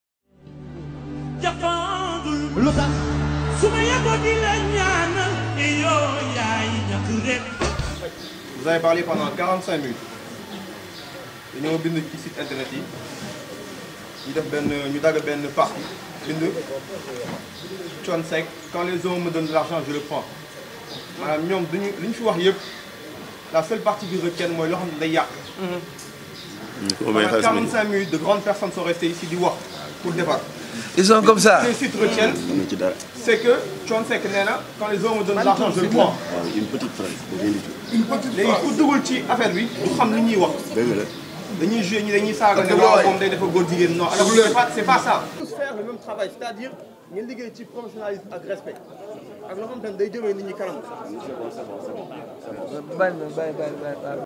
Polémique t-shirt LGBT: Seydina Aliou frère Wally s'attaque aux journalistes"soyez professionnelle"
Polémique t-shirt LGBT: Seydina Aliou frère de Wally s'attaque à la presse:"soyez professionnelle"